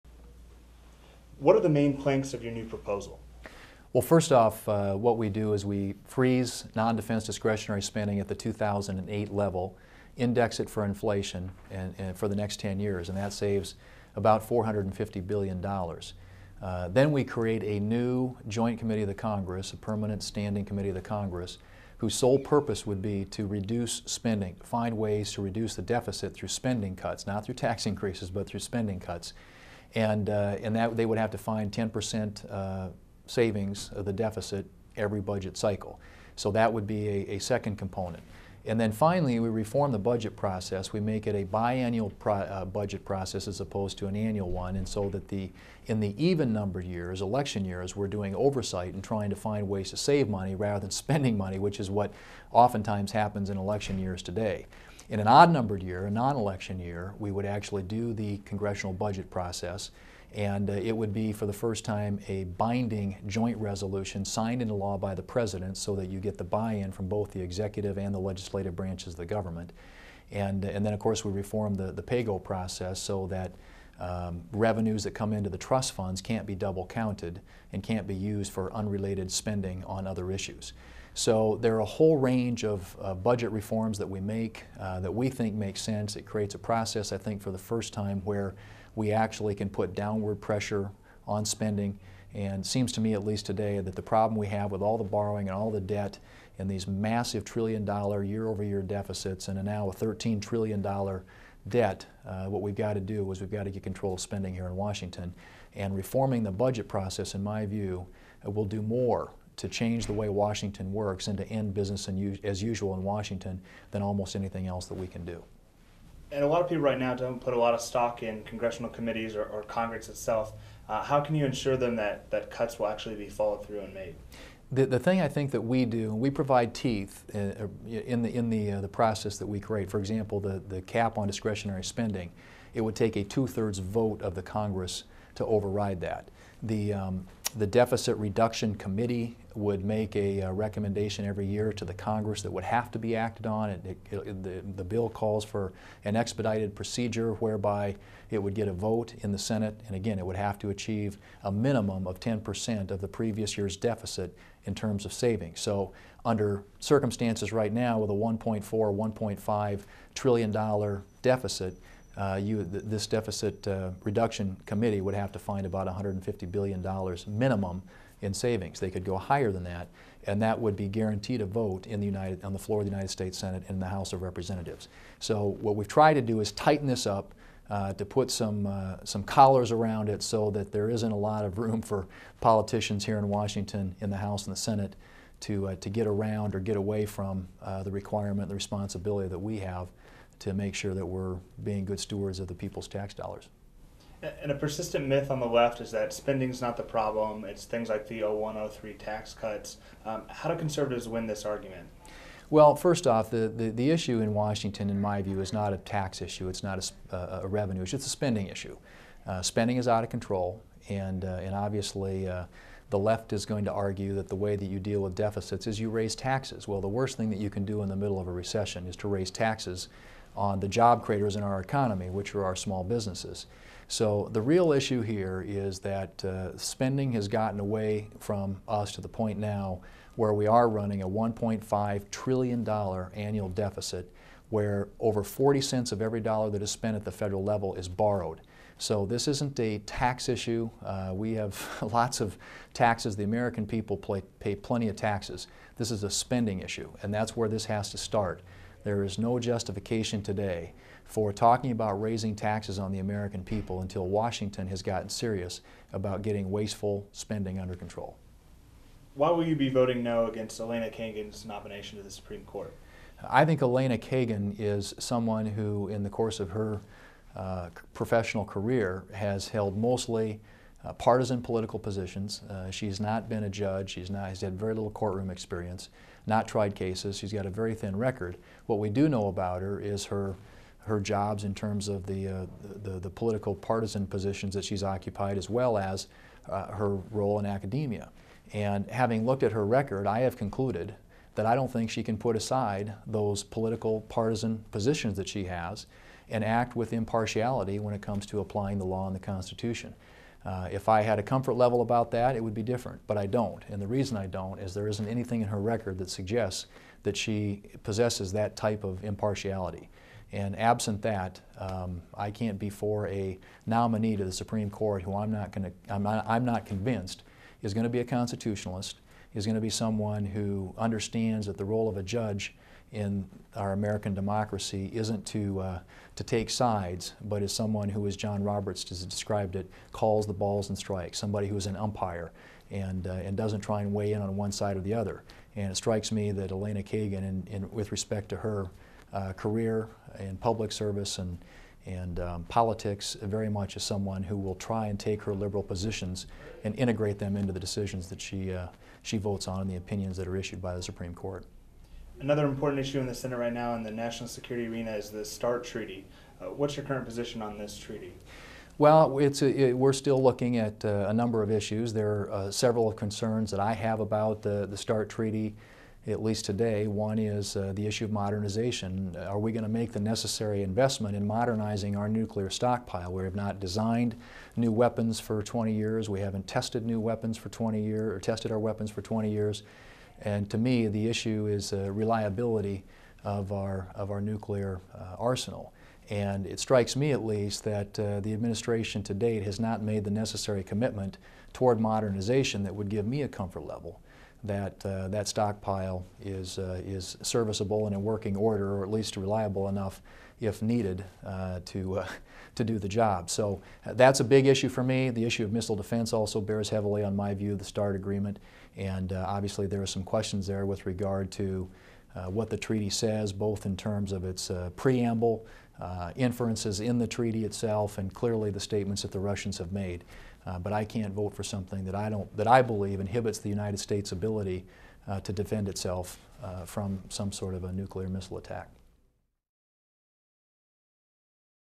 Senator John Thune Discusses Federal Spending, Elena Kagan, and the new START treaty
Senator John Thune (R-SD) spoke at The Heritage Foundation on the urgent need to rein in federal spending and reform the budget process. As Thune explained, there are 26 committees and subcommittees dedicated to spending, but not one to cutting spending.

Afterwards, he sat down with us for a short interview about his proposal, his position on the confirmation of Elena Kagan to the Supreme Court, and the new START treaty.